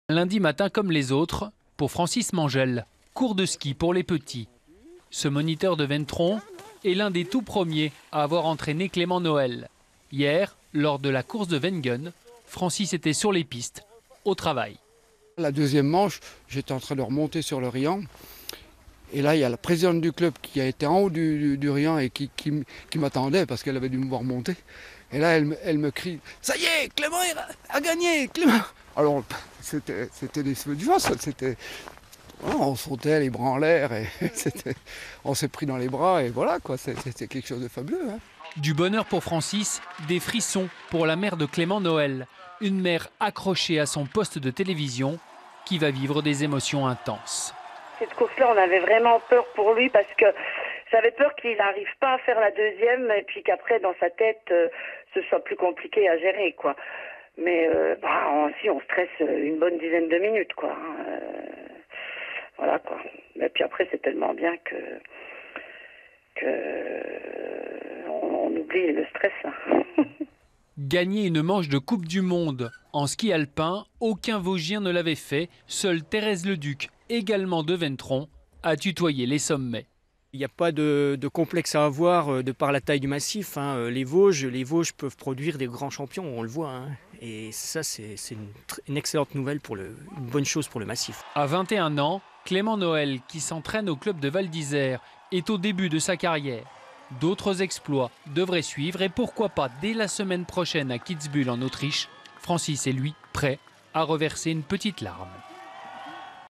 Réactions au Ventron après la victoire de Clément Noël lors d'une manche du championnat du monde
Clément Noël, originaire de Ventron a remporté une manche au championnat du monde de slalom à Wengen en Suisse. Réactions de ses proches dans sa contrée natale.
David Bailly - Jean-Pierre Petitcolas - Caroline Seytre.